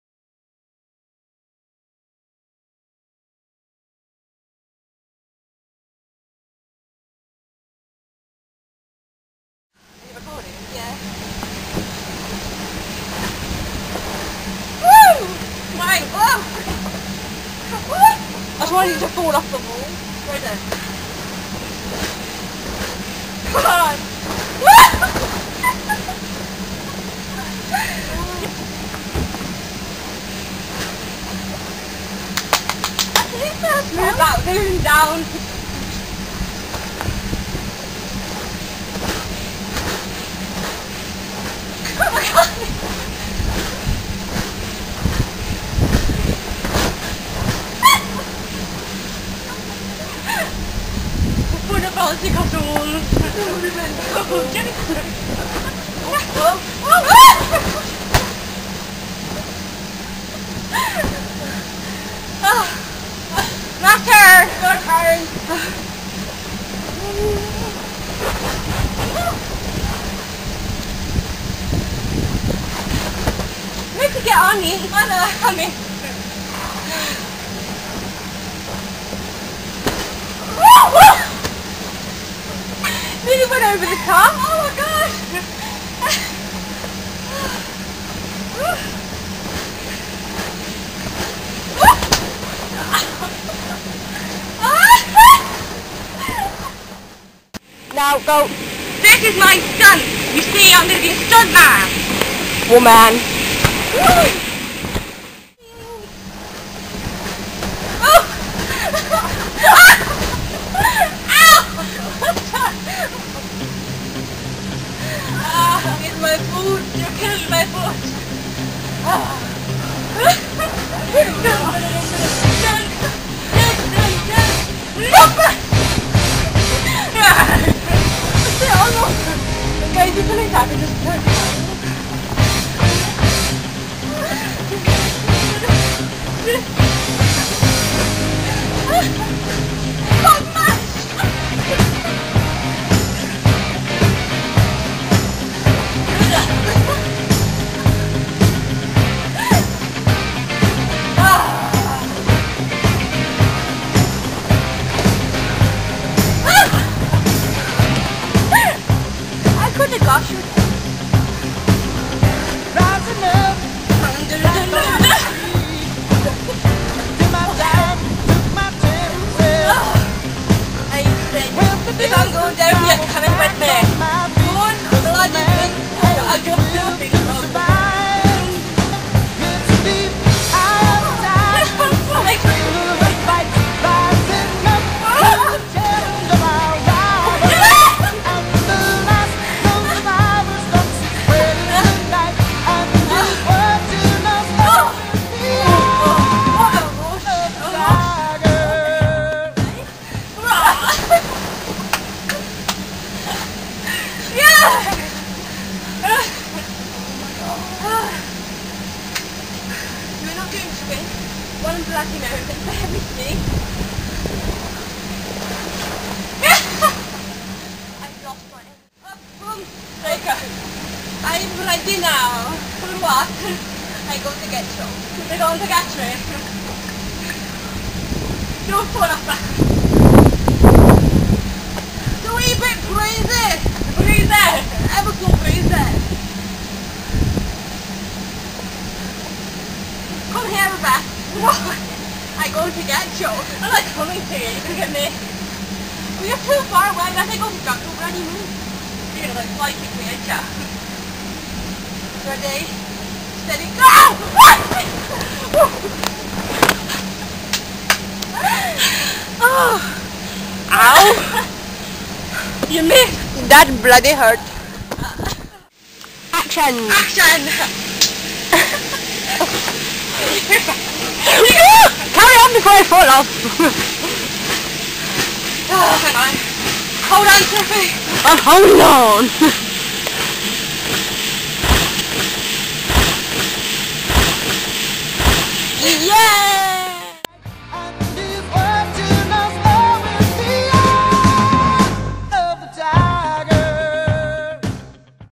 VLog 04 - Bouncy Castle (Vuh-Log)
This is what happens when Beth and Sophie are let loose on a bouncy castle...